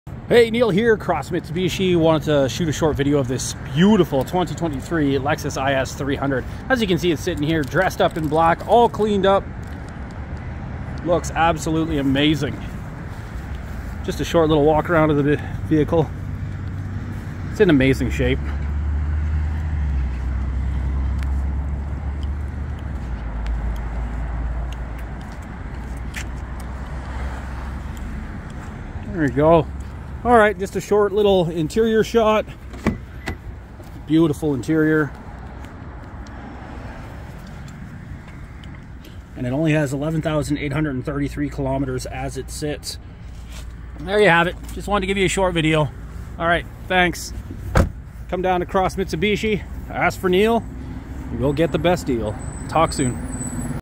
Awesome 2023 Lexus IS 300
Amazing one owner, accident free Lexus IS 300,
under 12,00km and in excellent condition.